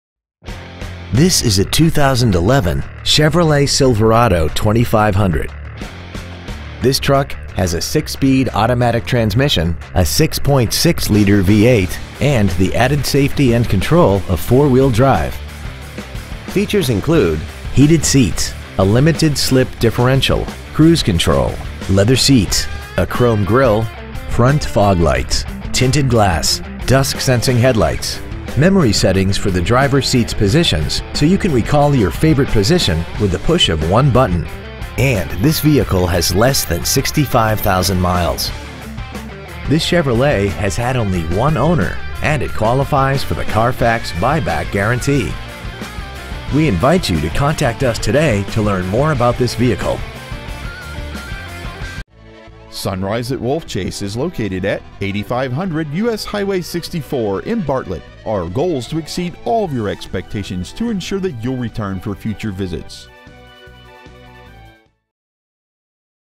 2011 Chevrolet Silverado 2500HD Bartlett TN
Year : 2011
 Make : Chevrolet
 Model : Silverado 2500HD
 Engine : 6.6L V8 Diesel
 Trans . : Automatic 6-Speed
 Exterior : Black
 Miles : 64,931

 Stock : G23602A